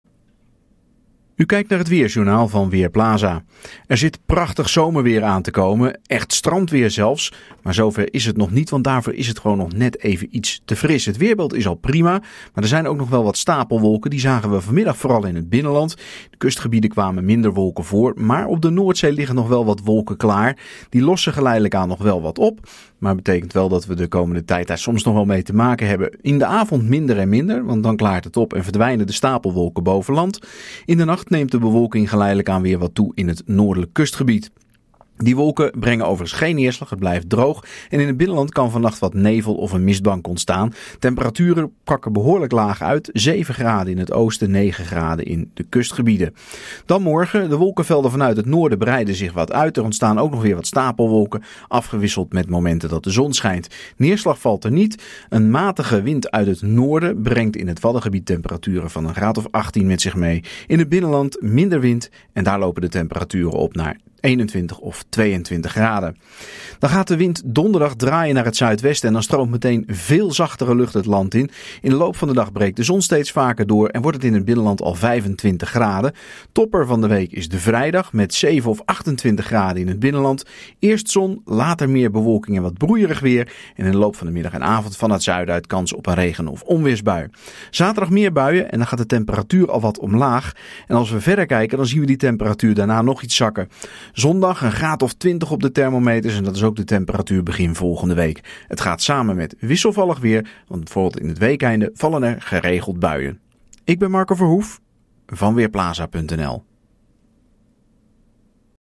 Weerbericht dinsdagmiddag: Warmer vanaf donderdag
Er wacht ons een frisse nacht, morgen wordt het 18 tot 22 graden. Zon en stapelwolken wisselen elkaar dan af en hte blijft droog. Vanaf donderdag is het warmer, vrijdag neemt de kans op onweer toe.